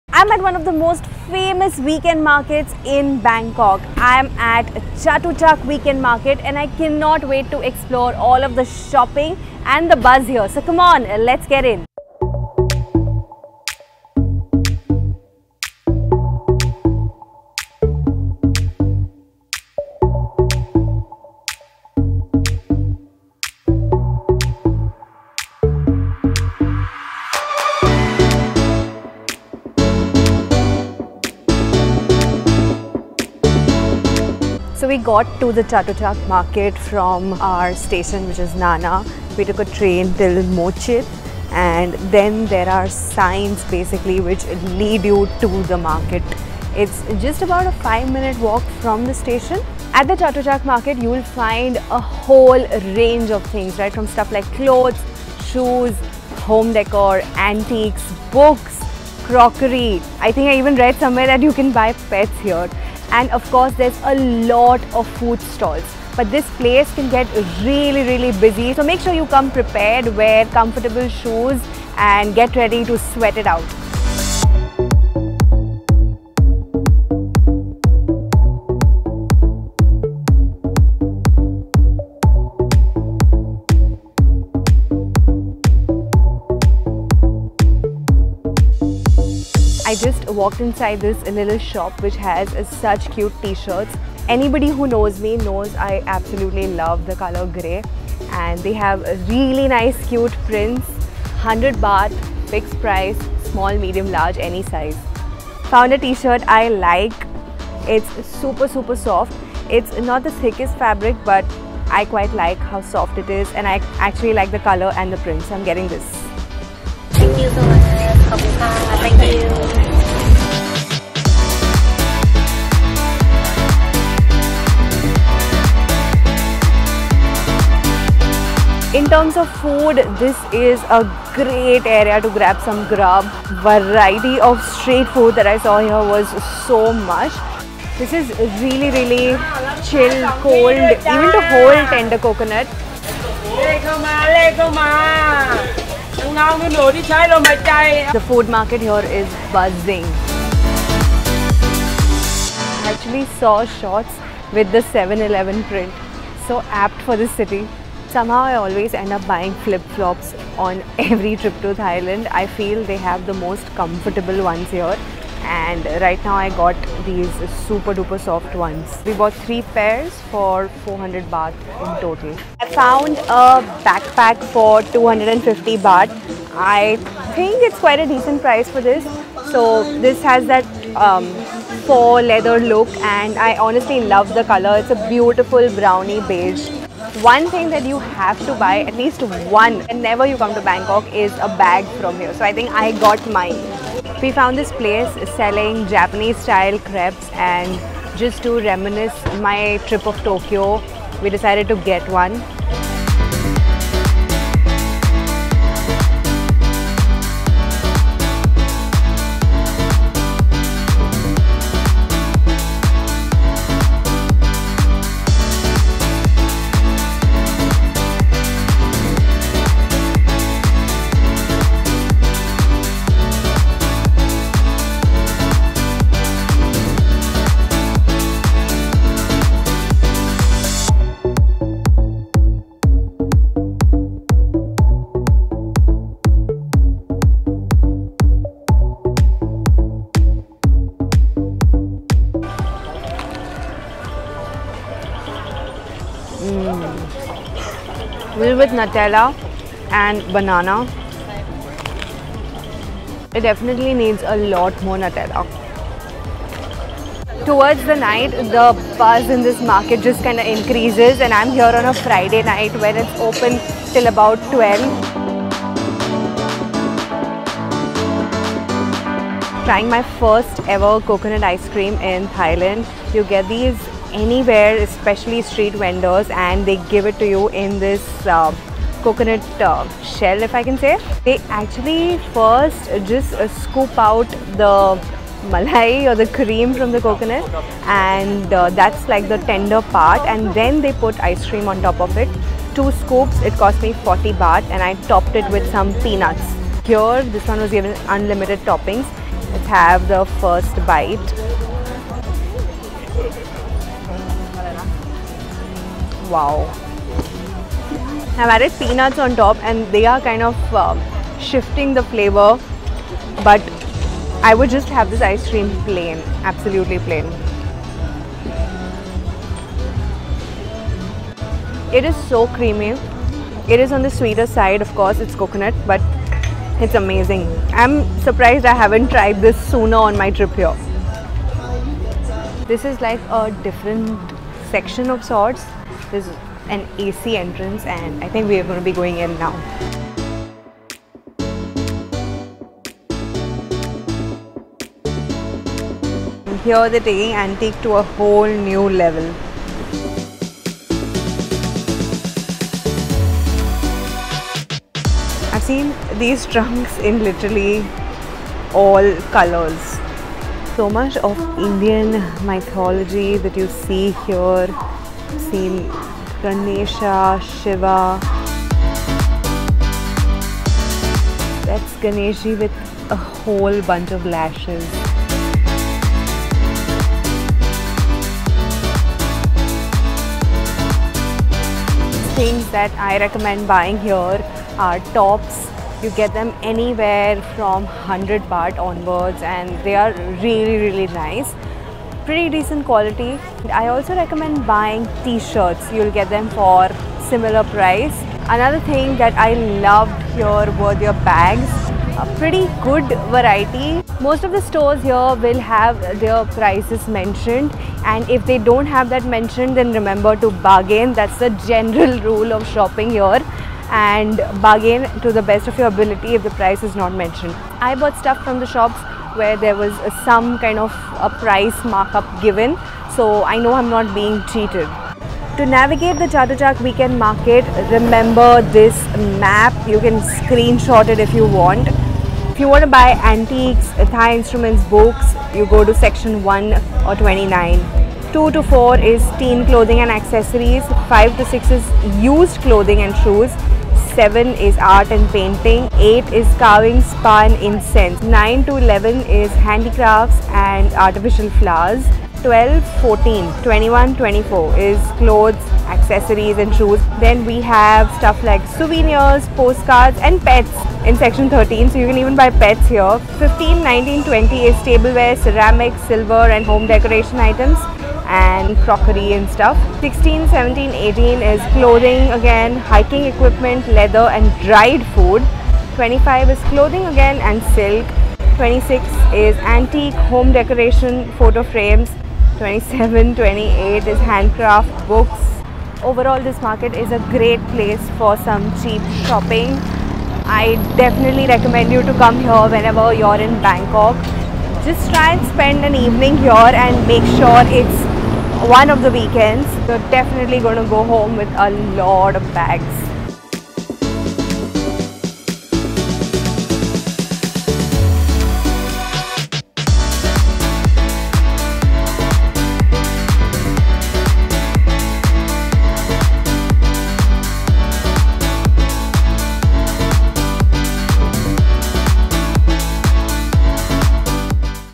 Bangkok Shopping & Street Food | CHATUCHAK WEEKEND MARKET
Chatuchak weekend market is one of the biggest shopping markets in Bangkok. Jatujak or Chatuchak is a must visit for every tourist. Whether you want to enjoy shopping or street food in Bangkok, this one is a must visit. To get to Chatuchak, head to Mo Chit Station via Bangkok BTS and then follow the signs to Chatuchak market. Here are the timings:
Friday - 6pm - 12:00 am
Saturday & Sunday - 9am - 6pm
Chatuchak Weekend Market, สวนจตุจักร Kamphaeng Phet 2 Rd, Chatuchak, Bangkok 10900, Thailand

At Chatuchak, I recommend buying the following:
Tops & Tshirts
Souvenirs
Bags - Backpacks and Sling Bags
And if you love local street food then there is so much of Thai street food that you can try here. If you are a vegetarian though, options are much limited

In terms of Shopping, Bangkok is really good. Most of the shopping in bangkok is already very affordable, but at Chatuchak you can find great wholesale and retail rates.

My Filming Equipment:

Canon M50

Canon 700 D camera

Iphone X

Lapel Mic